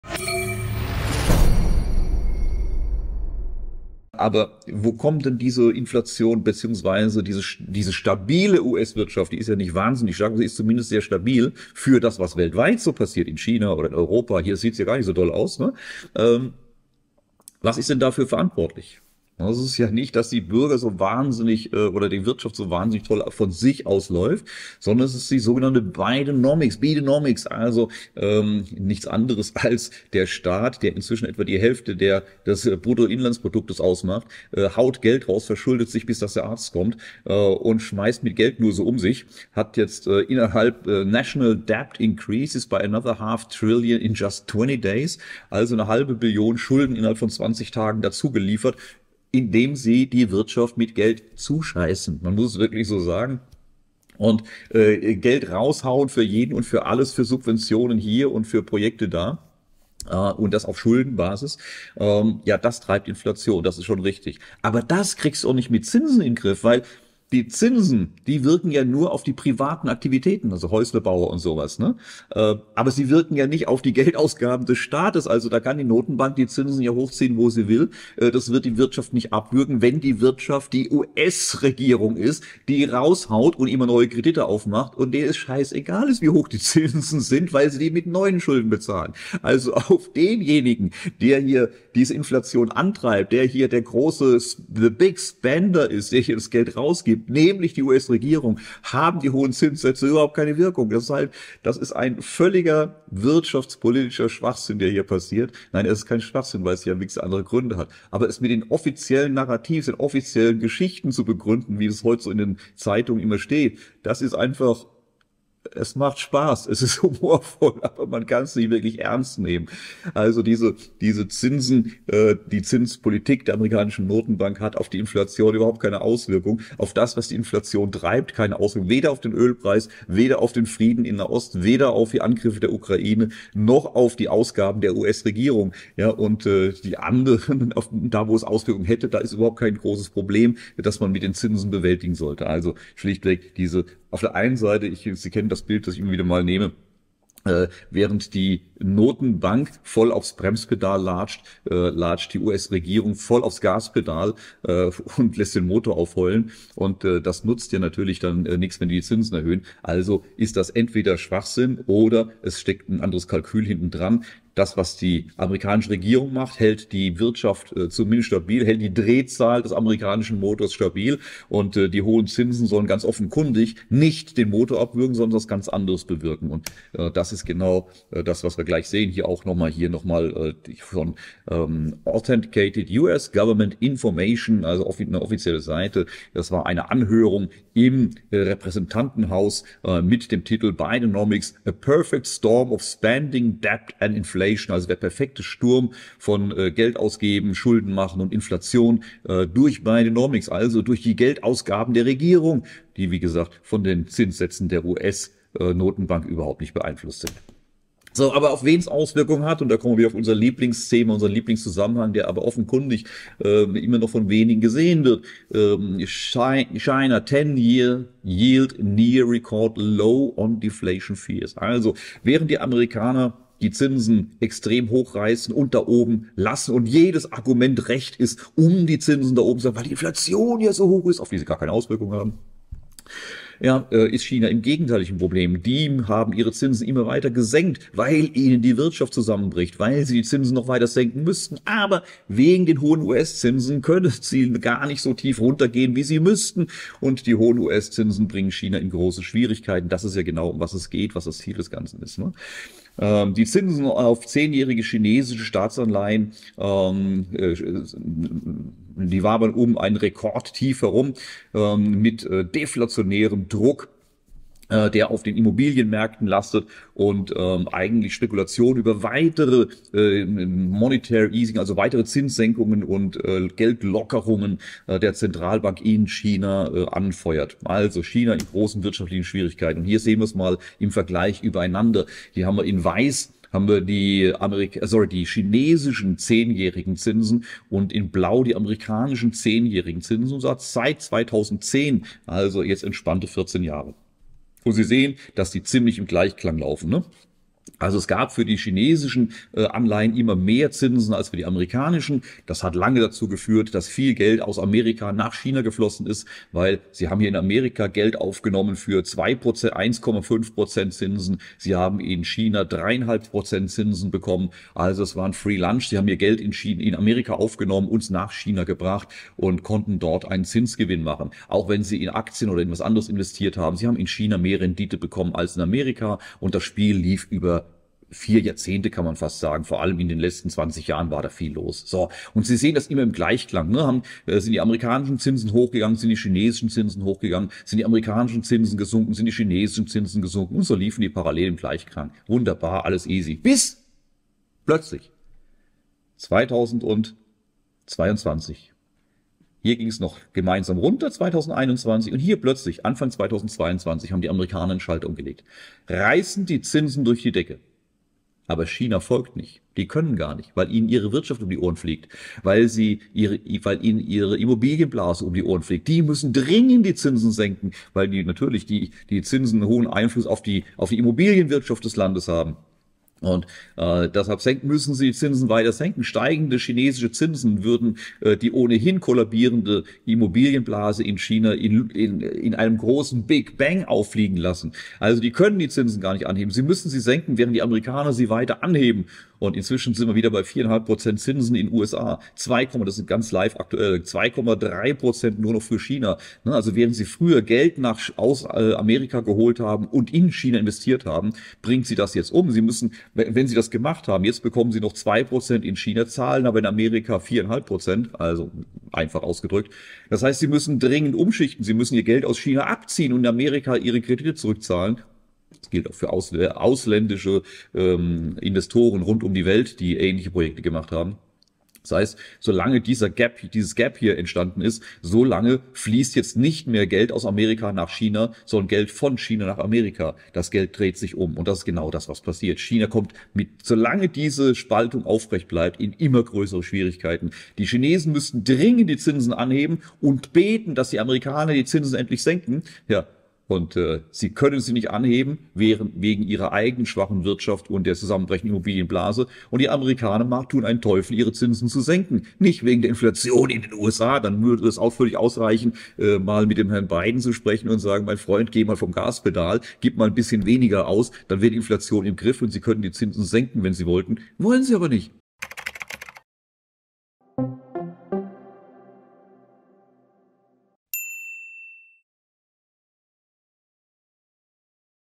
Dirk Müller - Falsche Narrative! DARUM bleiben die Zinsen hoch, bis...
𝐔𝐧𝐬𝐞𝐫 𝐊𝐨𝐧𝐠𝐫𝐞𝐬𝐬 𝟐𝟎𝟐𝟒 𝐚𝐦 𝟎𝟕.𝟎𝟗.𝟐𝟎𝟐𝟒.

(Bei diesem Video handelt es sich um einen kurzen Ausschnitt aus dem Marktupdate vom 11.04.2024 auf Cashkurs.com.)

Bildrechte: Cashkurs.com / Domenico Fornas - Shutterstock.com

– Ihre unabhängige Finanzinformationsplattform zu den Themen Börse, Wirtschaft, Finanzmarkt von und mit Dirk Müller